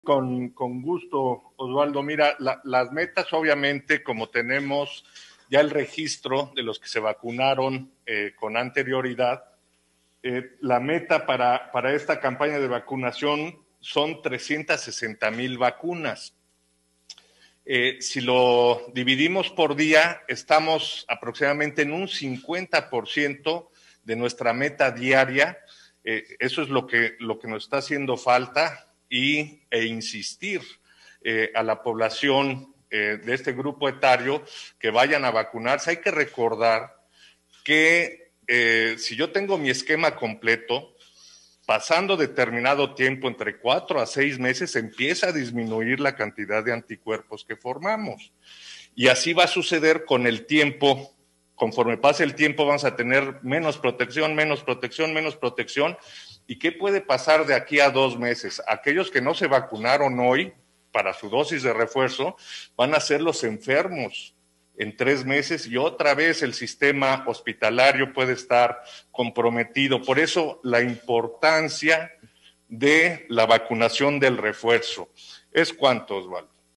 La población de 18 a 29 años de Puebla capital no está acudiendo por su tercera dosis anticovid
El titular de Secretaría de Salud, José Antonio Martínez García, señaló que la gran mayoría de los mayores de 18 años no han acudido a aplicarse su refuerzo contra la COVID19, ya que hay registros del 50% de la capacidad de la vacunación diaria.💉